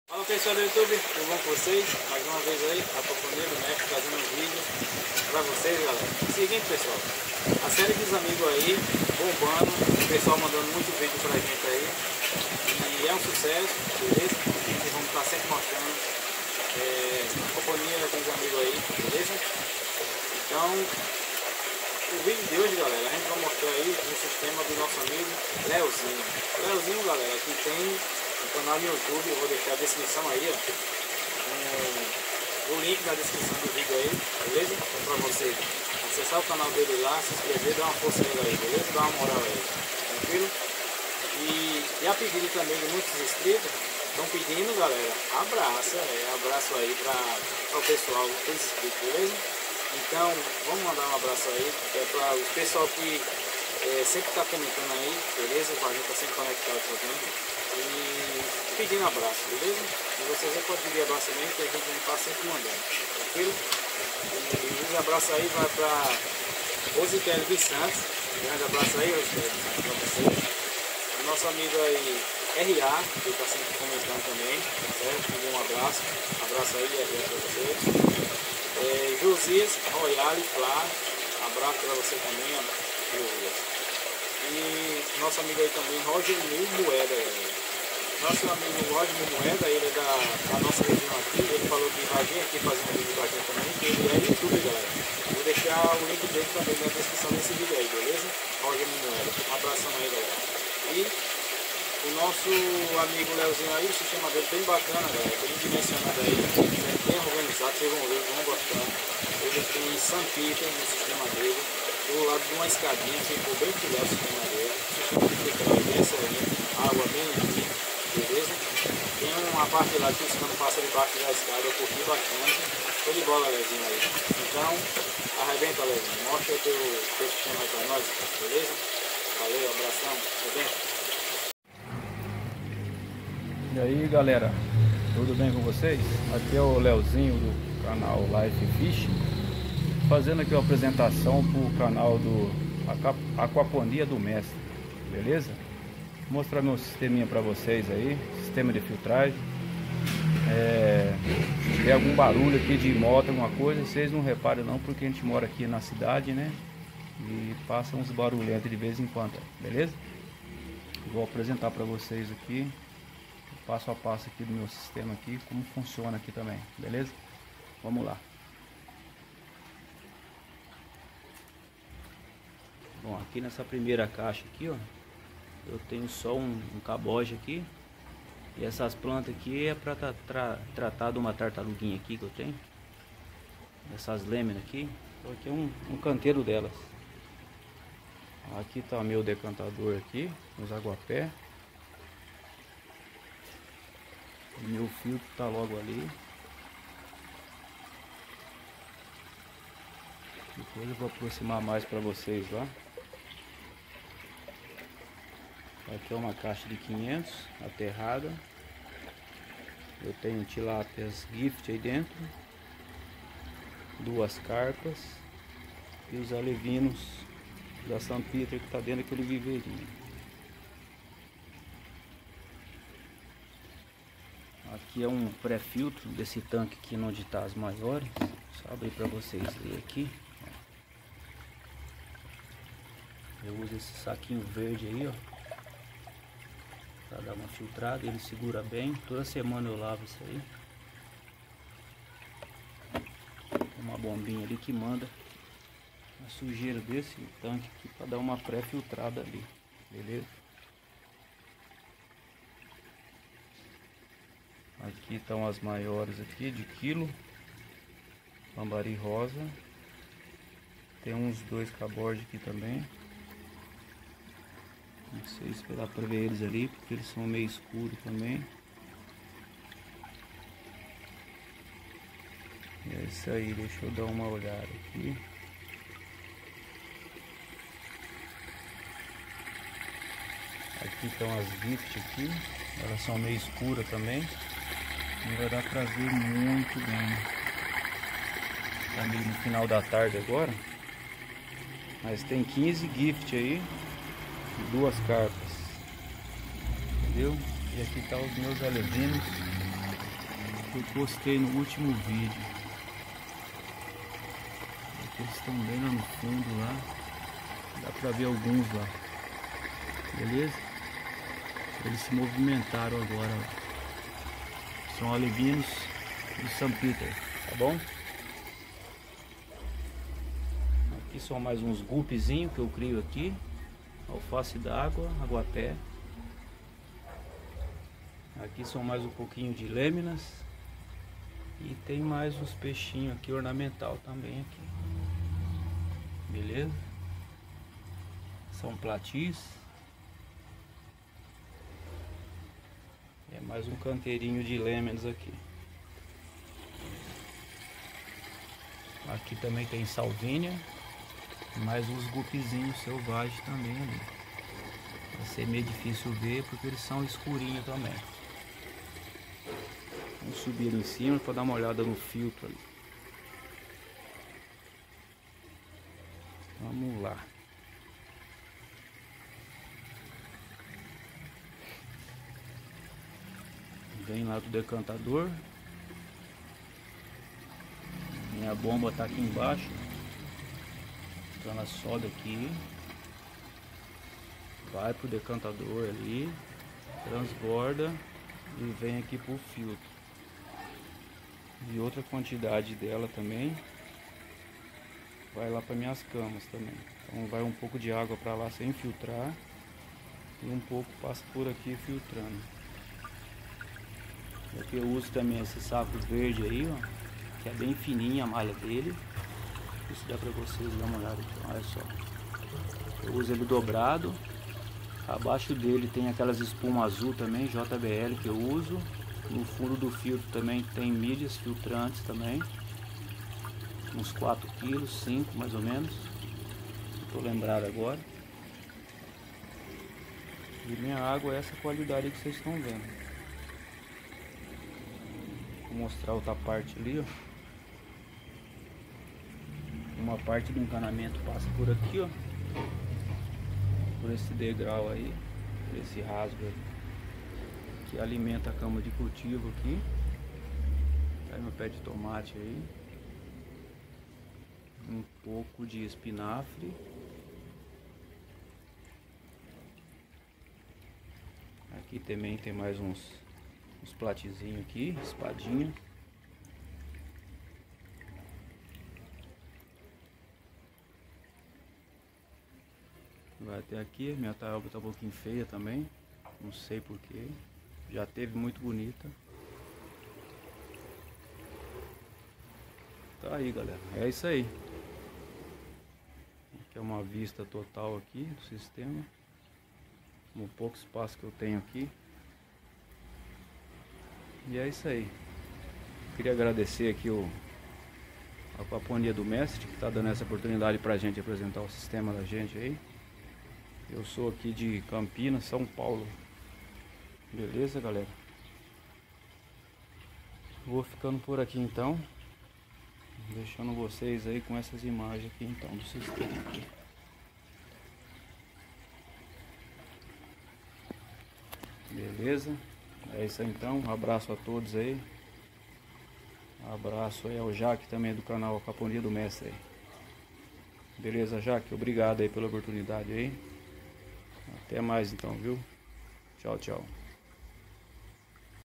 serie amigos do mestre criando peixe tilapia em caixa d'água #02 sistema do amigo leozinho.
link do canal life & fish👇👇👇🐟🌿😃

link do canal roger moore moedas👇👇👇💰💸🔎

uma técnica bastante utilizada no extremo Oriente.

na combinação de criação de peixes,camarões, lagosta e fechando o ciclo com frutas frutiferas, hortaliças e várias plantas.

Ao decorrer do tempo vamos subir vários vídeos aqui na plataforma e fotos no instagram abaixo.

uma tecnica moderna e sustentável que está ao tempo todo se transformando e trazendo novidades.

deixa teu like e não esqueça de se inscrever e seguir o insta ok😇🙏